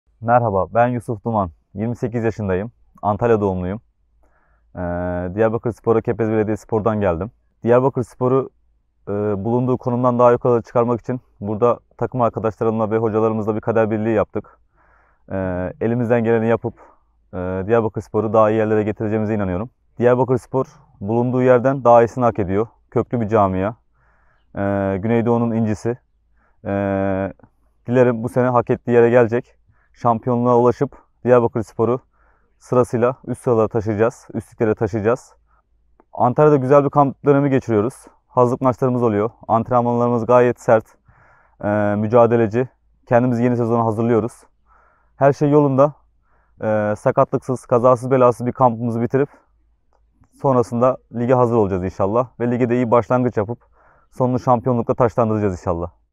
TAKIMIMIZI TANIYALIM 🎬Sağ, Sol Kanat: Yusuf DUMAN
📜TAKIMIMIZI TANIYALIM 🔎

- Adı Soyadı: Yusuf DUMAN
- Mevki: Sağ, Sol Kanat
- Yaş: 28
- Geldiği Takım: Kepez Belediyespor

Takımımızı tanıyalım serisinde kameralarımızın karşısına Yusuf DUMAN geçti! 🎬

Yusuf ve diğer futbolcularımızın tanıtım videolarını kulübümüzün resmi YouTube kanalından izleyebilirsiniz! ✅⚡